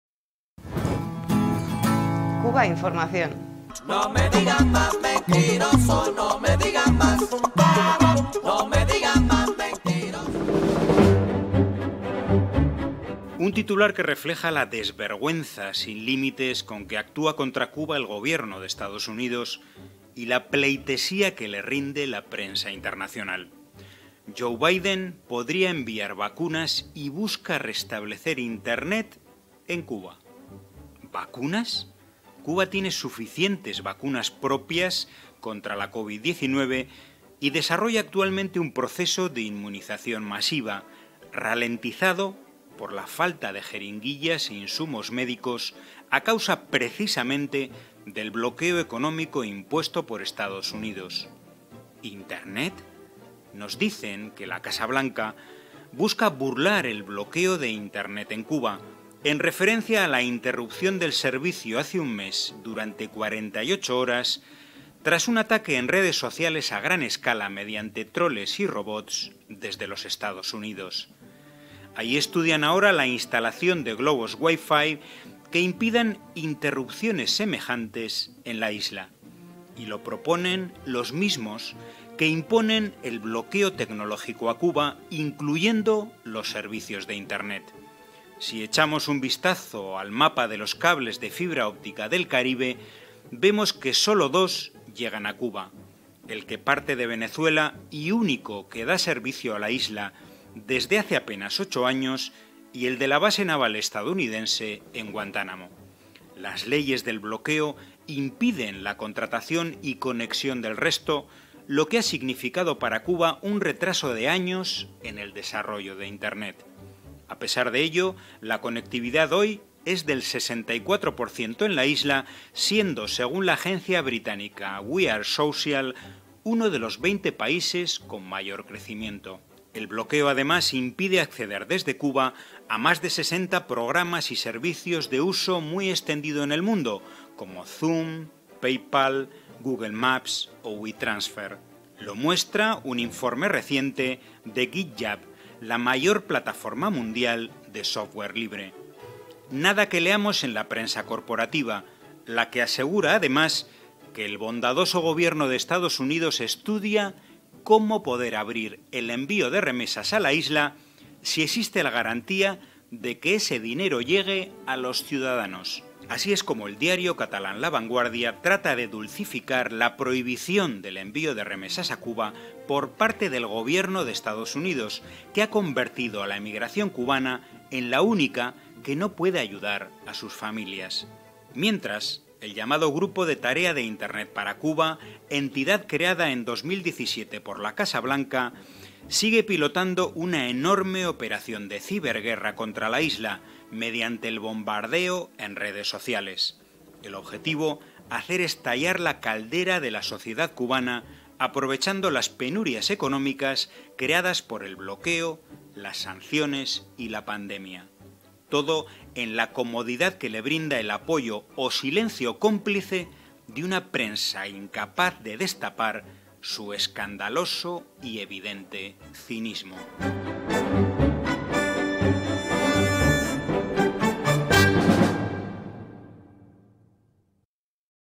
Cubainformacion TV: Internet, Impfstoffe, Auslandsüberweisungen : USA versprechen, was sie verbieten
Internet, Impfstoffe und Überweisungen aus dem Ausland: Die USA versprechen, was sie verbieten

José Manzaneda, Koordinator von Cubainformación. Eine Titelzeile spiegelt die grenzenlose Unverschämtheit wieder, mit der die Regierung der Vereinigten Staaten gegen Kuba agiert und mit der die internationale Presse dieser huldigt: „Joe Biden (…) könnte Impfstoffe schicken und ist bestrebt, das Internet in Kuba wieder herzustellen“ (1).

Impfstoffe? Kuba hat genügend eigene Impfstoffe gegen Covid 19 und führt gegenwärtig einen Prozess der massiven Immunisierung durch (2), der allerdings verlangsamt wird  durch das Fehlen von Spritzen und medizinischem Material – und das wird verursacht – ganz genau – durch die ökonomische Blockade der USA (3).

Internet? Man sagt uns, dass das Weiße Haus „bemüht ist, die Blockade des Internets in Kuba zu umgehen“ und bezieht sich dabei auf die Unterbrechung dieses Dienstes vor einem Monat für 48 Stunden, nach einem Angriff großen Ausmaßes in den sozialen Netzwerken mittels Trolls und Robotern von den Vereinigten Staaten aus (4).

Dort studiert man gerade die Installation von WiFi-Ballonen, die solche Unterbrechungen auf der Insel verhindern könnten (5). Und das schlagen Dieselben vor, die eine technologische Blockade einschließlich der Internetdienste  gegen Kuba verhängt haben (6). Wenn wir einen Blick auf die Karte der Glasfaserkabel der Karibik werfen, sehen wir, dass nur zwei Kuba erreichen: dasjenige, das aus Venezuela kommt und das seit kaum 8 Jahren das einzige zur Versorgung der Insel mit diesen Diensten ist, und dasjenige zur US-amerikanischen Marinebasis in Guantánamo (7). Die Blockade-Gesetze verhindern einen weiteren Vertragsabschluss und die Anbindung, was für Kuba einen Rückstand von Jahren bei der Entwicklung des Internets bedeutet. Trotzdem liegt die Anbindung der Insel heute bei 64%, womit Kuba  nach der britischen Agentur „We Are Social“ zu den 20 Ländern mit den größten Wachstumsraten gehört (8).

Die Blockade verhindert außerdem den Zugang von Kuba aus zu mehr als 60 Programmen und Dienstes, deren Nutzung in der Welt am meisten verbreitet ist: Zoom, PayPal, Google Maps oder Wetransfer. Dies zeigte ein kürzlicher Bericht von GitHub, der weltgrößten Plattform für freie Software (9).

Davon lesen wir allerdings nichts in der korporativen Presse. Das was sie aber versichert, ist, dass die gütige Regierung der Vereinigten Staaten Möglichkeiten sucht, wie sie den Versand von Geldzahlungen auf die Insel öffnen könnte, wenn es eine Garantie gäbe, dass dieses Geld die Bürger erreichte (10). Auf diese Weise versucht die katalanische Tageszeitung La Vanguardia das Verbot der US-Regierung von Geldüberweisungen nach Kuba zu versüßen, eine Maßnahme, die die kubanische Migration zu der einzigen macht, die ihre Familien nicht unterstützen kann (11).

Währenddessen fährt die sogenannte Gruppe Aufgabe Internet für Kuba (Internet Task Force), die 2017 vom Weißen Haus gegründete Einheit, fort, eine gewaltige Operation des Cyberkrieges gegen die Insel zu pilotieren, mittels einer Bombardierung in den Sozialen Netzwerken. Das Ziel: einen Hexenkessel in der kubanischen Gesellschaft zu schaffen, wobei die ökonomischen Engpässe ausgenutzt werden, die von der Blockade, den Sanktionen und der Pandemie verursacht wurden (13).

Und das alles begleitet von der bequemen Unterstützung – oder dem komplizenhaften Schweigen – einer Presse, die unfähig ist, diesen skandalösen und offensichtlichen Zynismus aufzudecken.